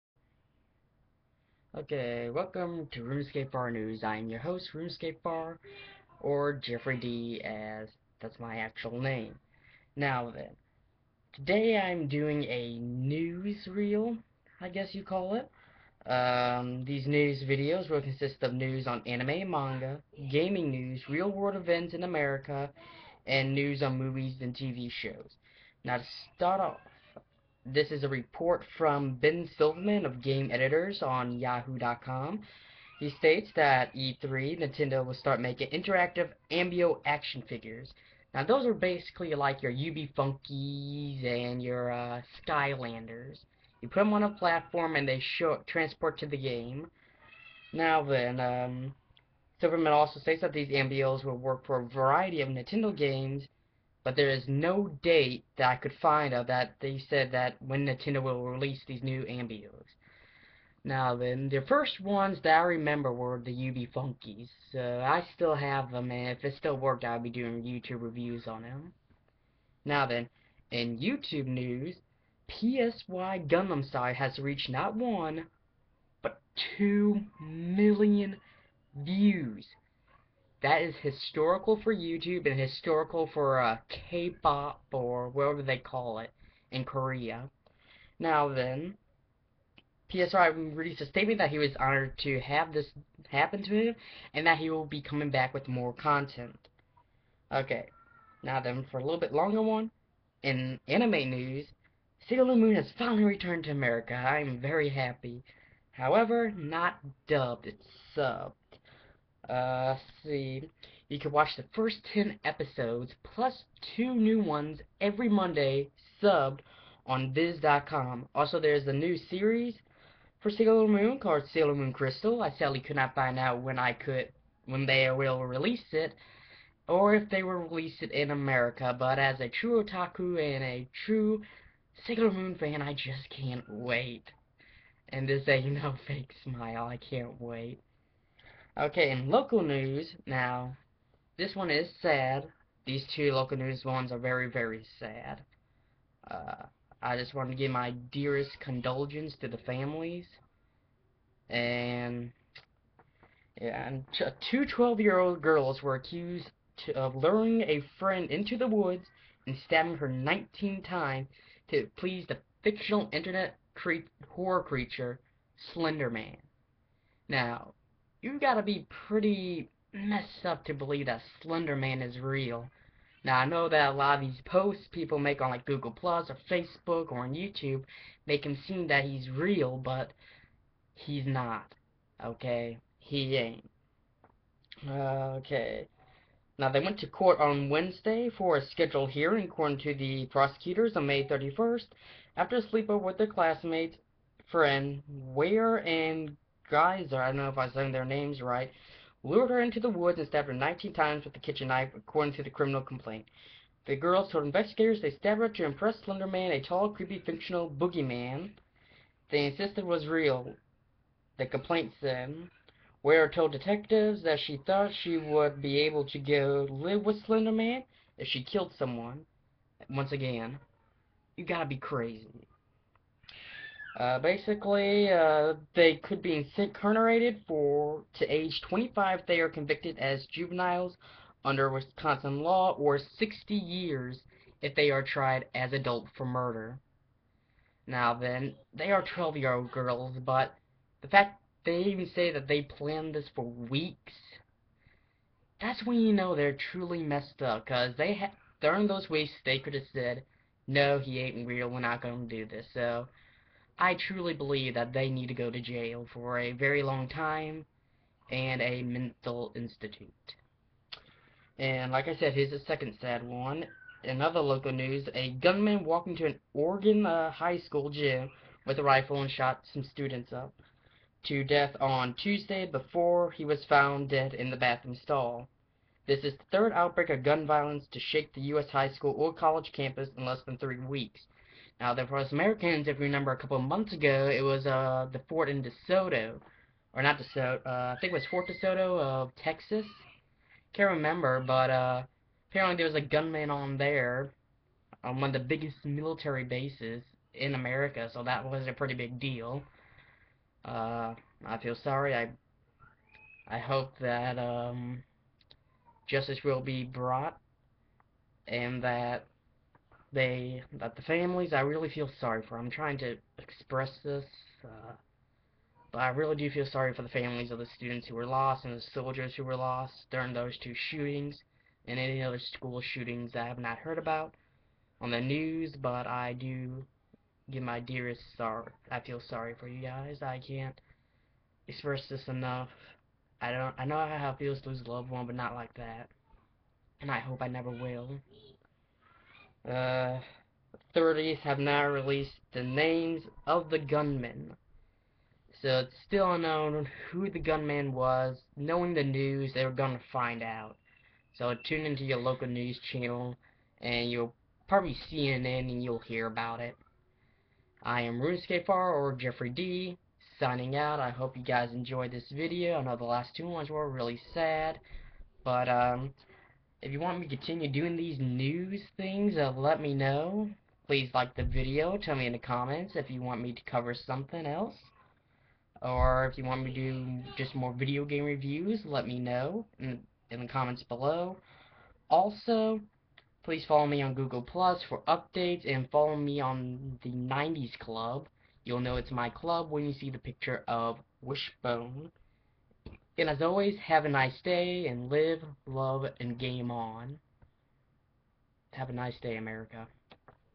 Runescapefar news brodcast #1 June 11, 2014 10:01 AM
to sum this up this is my first news broadcast and if you guys like this then let me know in the comments bellow this is something I have always wanted to do. again I pray for the family's and victims of the school shootings  and the victim of the slender man incident and hope that nothing like this will ever happen again.